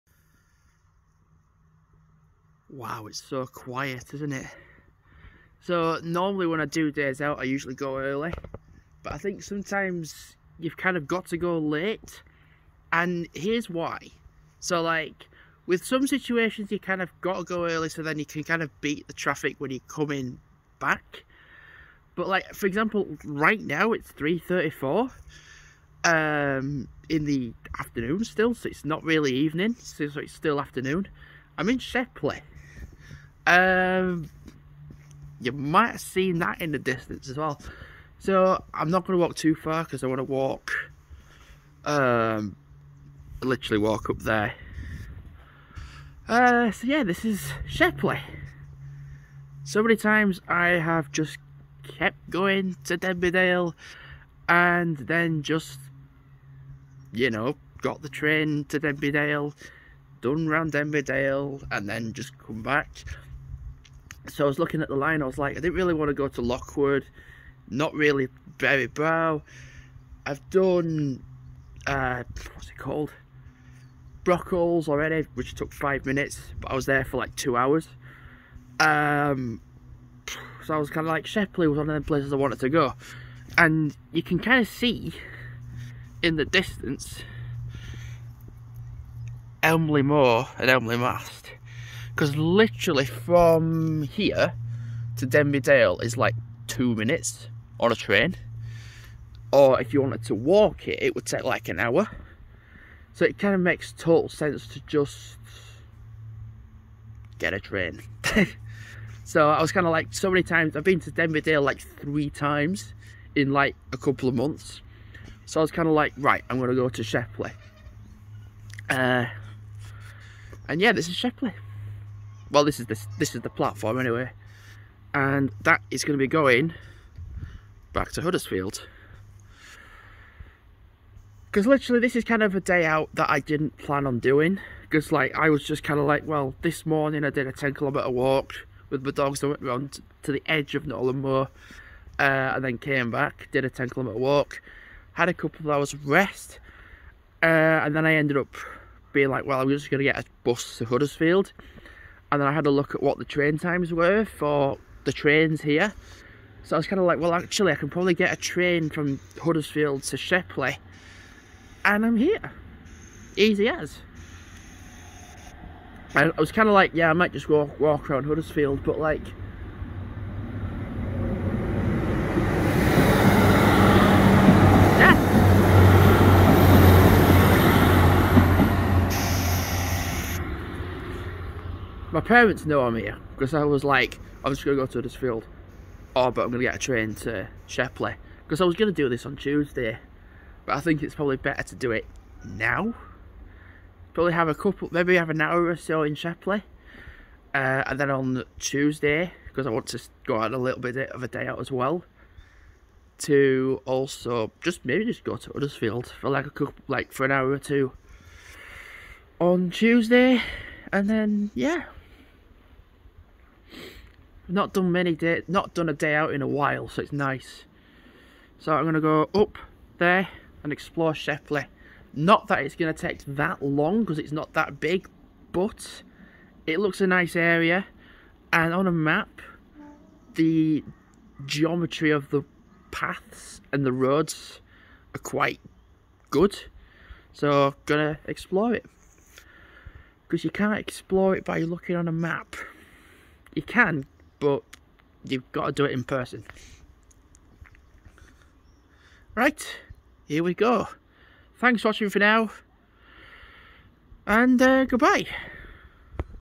SHEPLEY ARRIVAL VLOG
Hello Jamie here and with another video. Arriving at Shepley Platform, recording a little vlog before going on the explore, a nice and clean platform, not too far away from Denby Dale either.